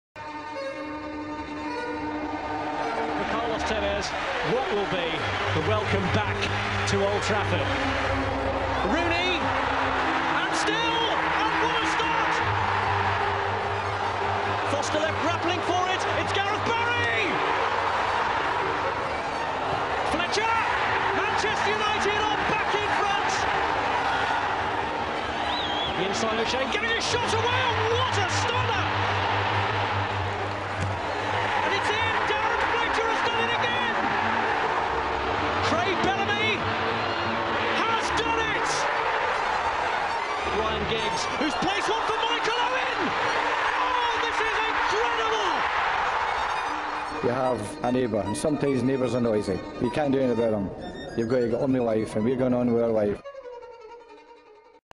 The Greatest Manchester Derby - SoccerShouts.com HD 720p
The Manchester derby is the name given to football matches between Manchester City and Manchester United. The local derby centres on the City of Manchester and Greater Manchester with approximately four miles separating the clubs with City based in east Manchester at the City of Manchester Stadium and United south west of Manchester at Old Trafford in Trafford. The relationship between both is portrayed as hostile, but incidents have united the city in the past.

Both have the two highest record home attendances in the history of English football -- City in 1934 with 84,569 and United with 83,260 in 1948, ironically at Maine Road due to damage to Old Trafford during the Second World War. Financially, both clubs' combined 2010 revenue of £410m (City -- £125 million and United -- £286 million) makes up nearly a quarter of the twenty-team Premier League revenue alone. The derby is also followed with interest nationally with 8.3 million people watching the first leg of the 2009--10 League Cup semi-final on television and over 10 million watching the 2010--11 FA Cup semi-final on terrestrial television